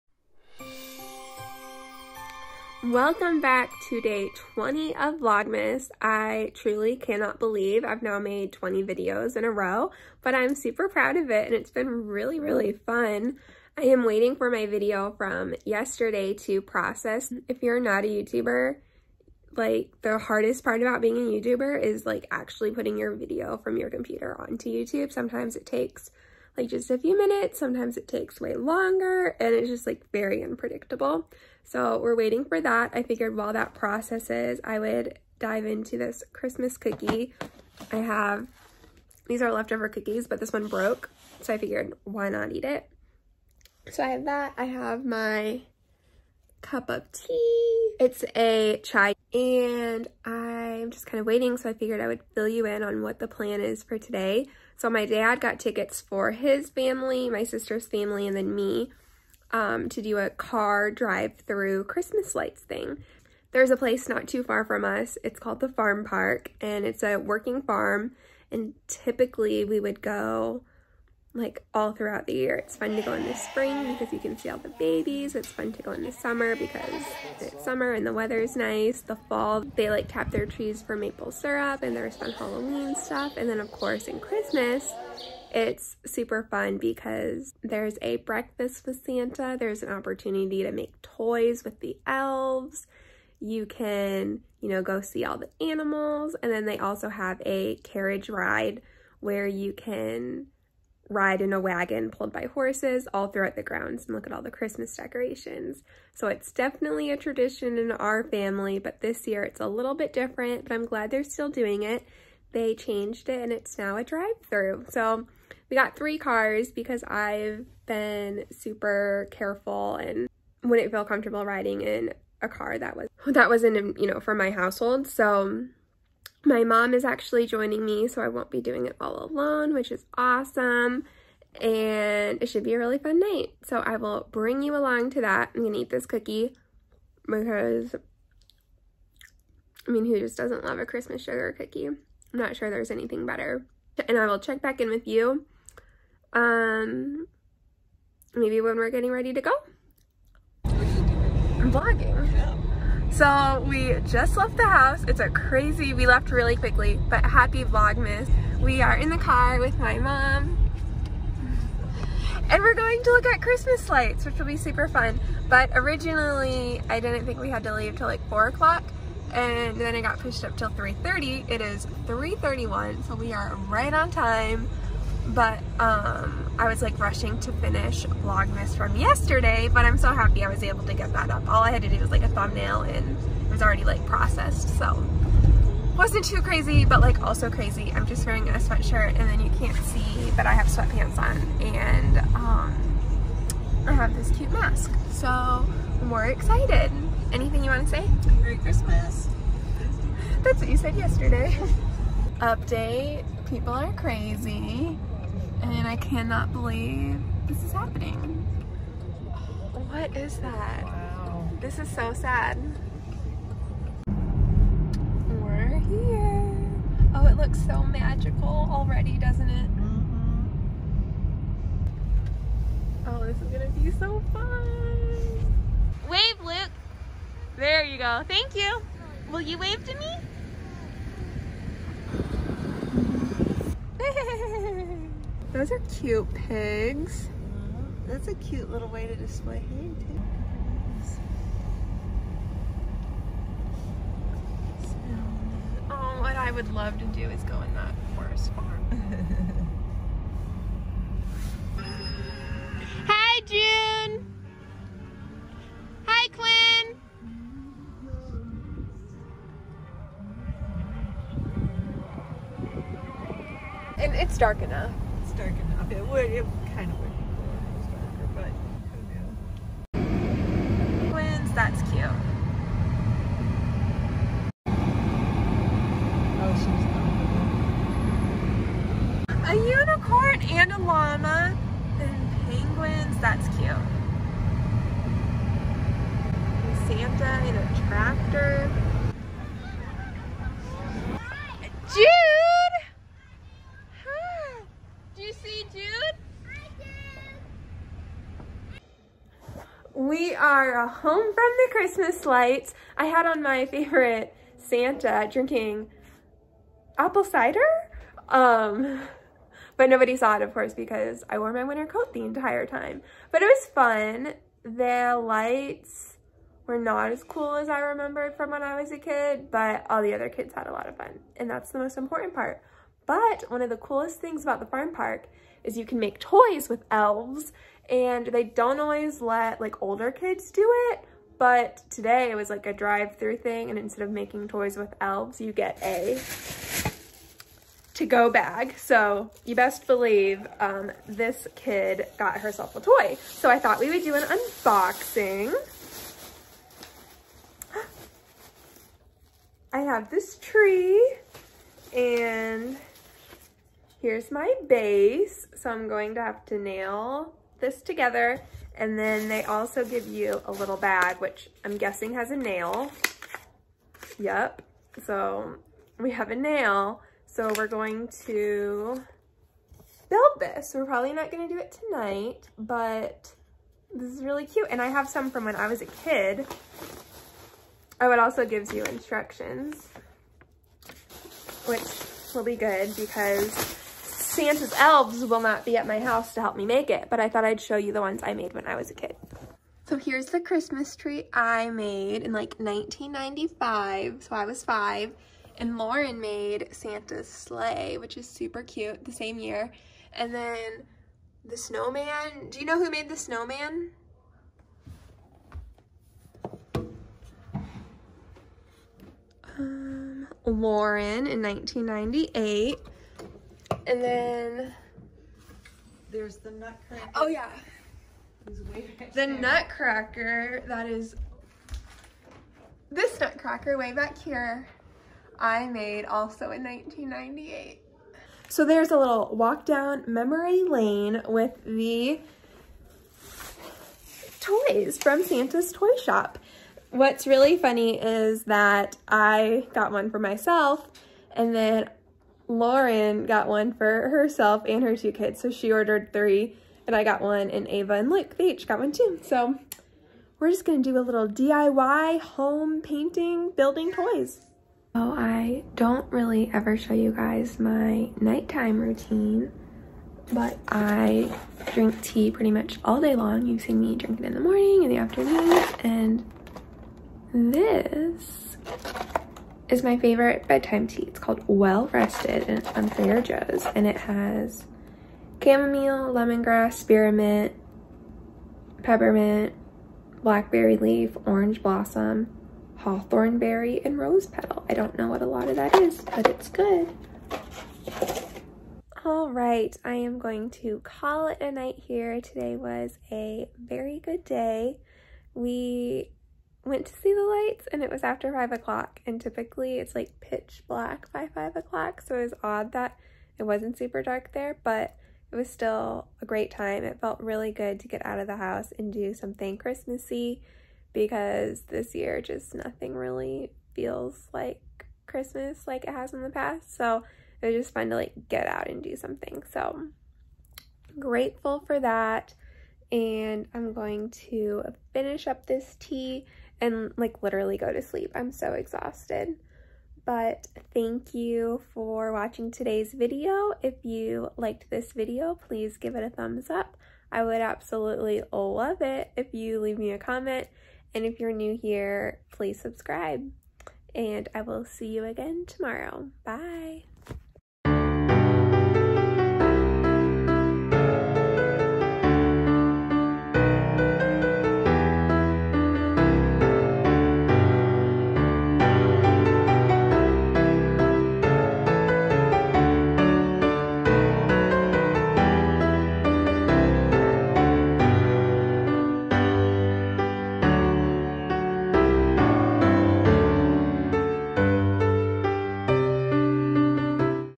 Drive-thru Christmas light show (vlogmas day 20)
Today my mom and I met up with some family to see the Country Lights at one of my favorite places, the Farmpark! It was a bummer to be in separate cars, but very special to do something kind of normal during this crazy year.

🧡💛🤍 Kay